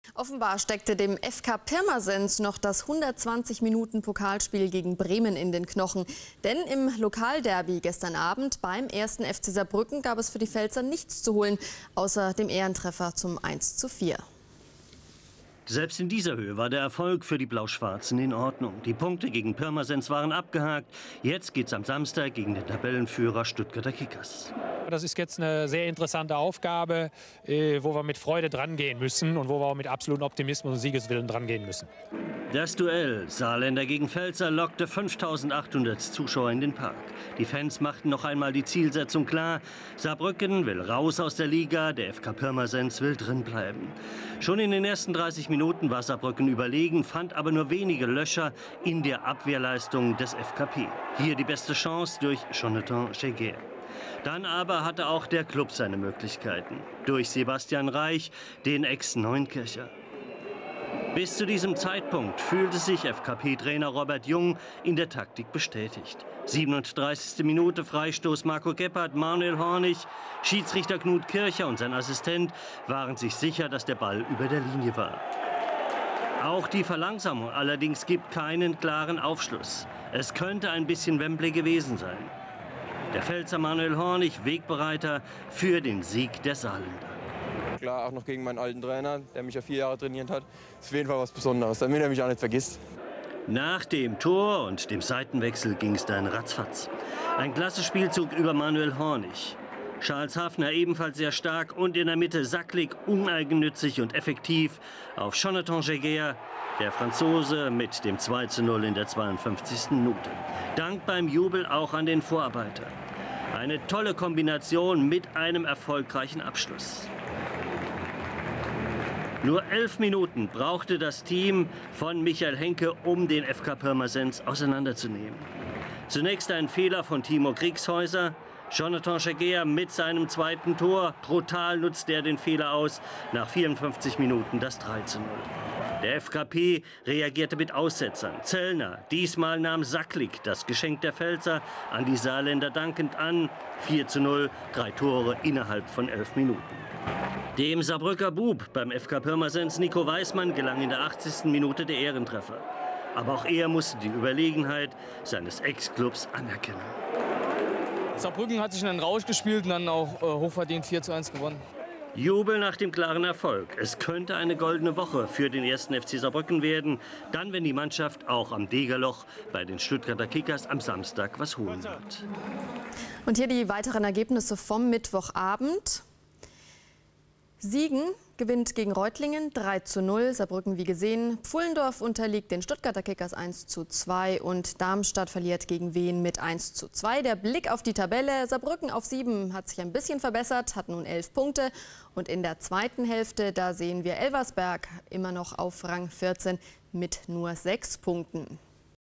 1.FC Saarbrücken - FK Pirmasens 4:1(1:0) --- 13.9.2006
1.FC Saarbrücken - FK Pirmasens 4:1(1:0)
Regionalliga Süd 2006/2007
7.Spieltag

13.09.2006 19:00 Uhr
Ludwigsparkstadion, Saarbrücken
Zuschauer: 5800
Schiedsrichter: Knut Kircher (Rottenburg)

1:0 Hornig (34.)
2:0 Jäger (51.)
3:0 Jäger (54.)
4:0 Saglik (62.)
4:1Weißmann (80.)

Gelbe Karten
Saarbrücken: -
Pirmasens: Lechner(1. Gelbe Karte),Hunsicker(1.)

1. FC Saarbrücken

Aufstellung:
Marina - Halet , Assoumani , Genet , S. Kling - Karaoglan , Hornig     , Haffner     , Gebhardt     - Jäger             , Saglik

Einwechslungen:
65. Laping für Haffner
65. Hajdarovic für Jäger
80. Reuter für Gebhardt

Trainer: Henke

FK Pirmasens

Aufstellung:
Steigelmann - Lechner     , T. Kriegshäuser , Hildebrandt , Falkenmayer - Zellner , Steil , Schaufler     , Weller     - Weißmann     , S. Reich

Einwechslungen:
46. Dengel für Schaufler
46. Hunsicker     für Weller
61. Akten für S. Reich

Trainer: Jung

SR aktueller bericht